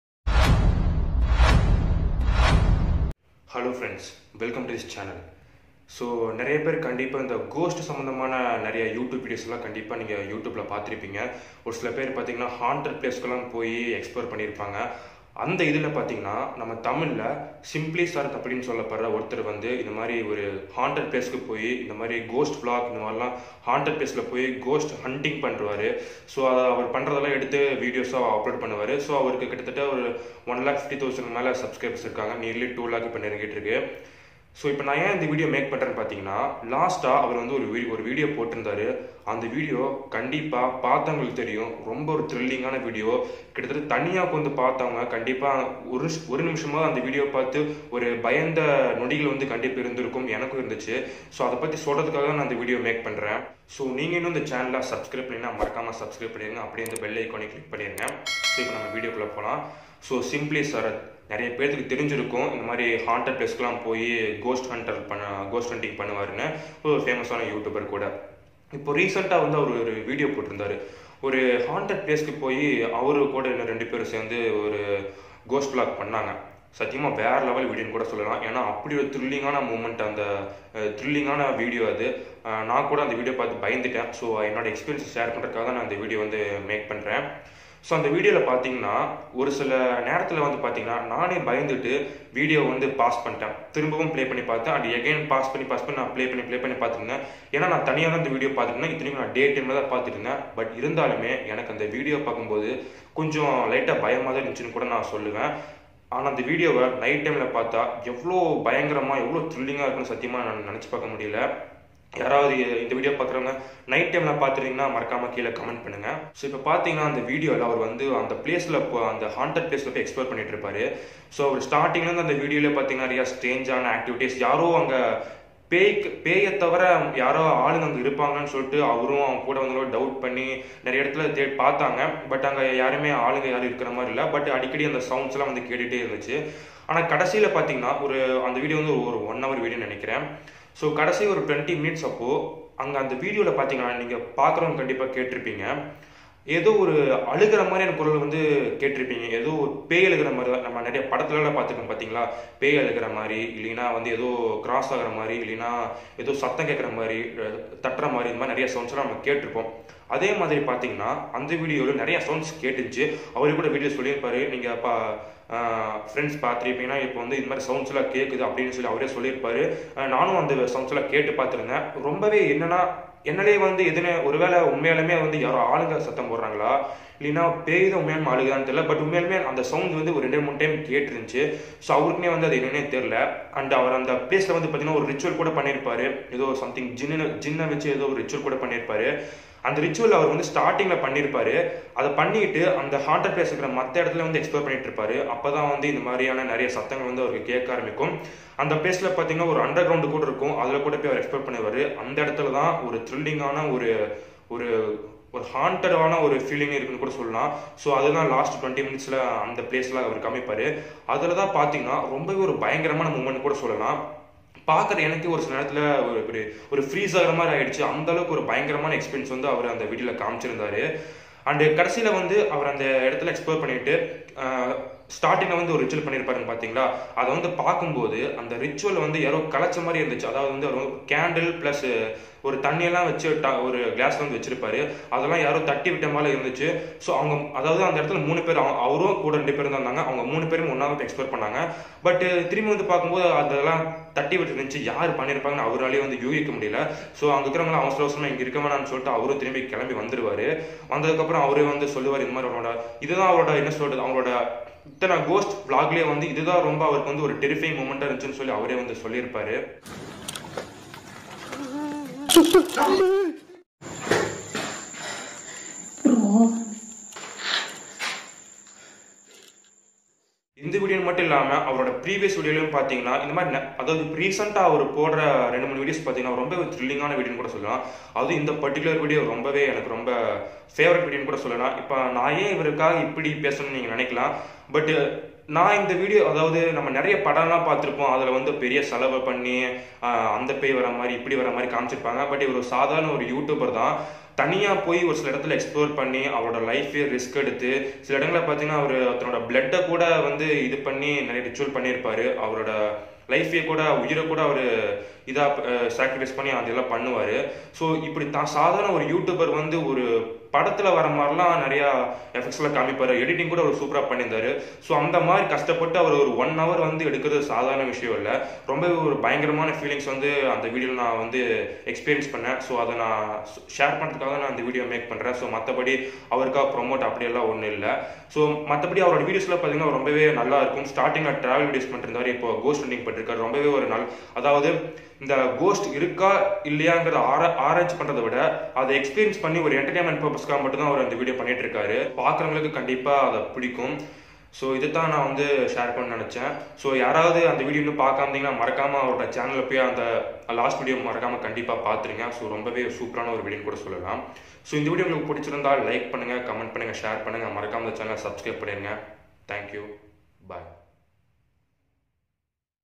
The End - Vera level ghost hunting by Simply Sarath | simply sarath ghost videos | Screen Play
Hi friends,

In this video I explained how Simply Sarath bro's recent video made me thrilled and it gave me haunted feelings. Vera level ghost hunting by Simply Sarath bro.

With Sarath bro permission I'm using his videos or pictures. So sarath bro fans don't mistake me I'm using without his permission.